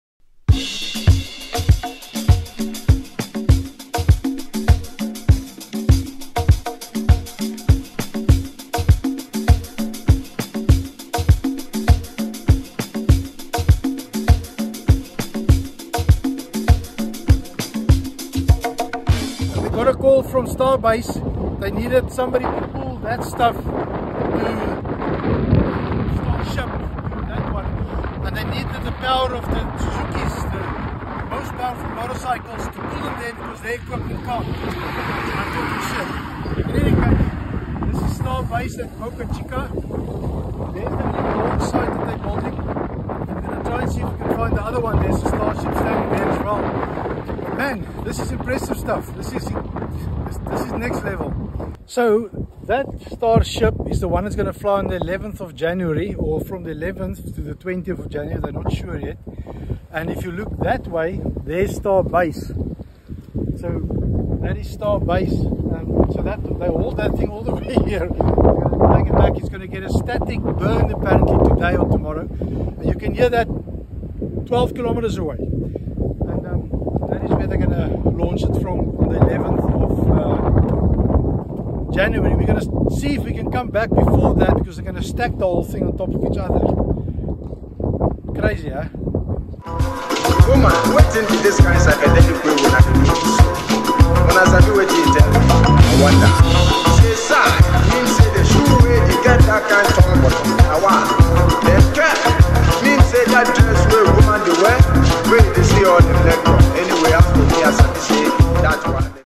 This is what Starbase SpaceX looks like at Boca Chica, Texas.
We visited Starbase SpaceX at Boca Chica in Texas.
It is located on the Usa Mexican border on the Gulf of Mexico.
It is just unbelievable how big the starship and rockets are. You can see on photos or video they’re big, but it really hits you when seeing them in real life.
The place they’re building here is also truly a very impressive setup. There’s loads of people still working on the buildings and launch site.
Unbelievable, we’re all so lucky to live in time of the moon landing and now this ambitious project.
There’s no visitors centre, yet, the rumours are there will be one in the near future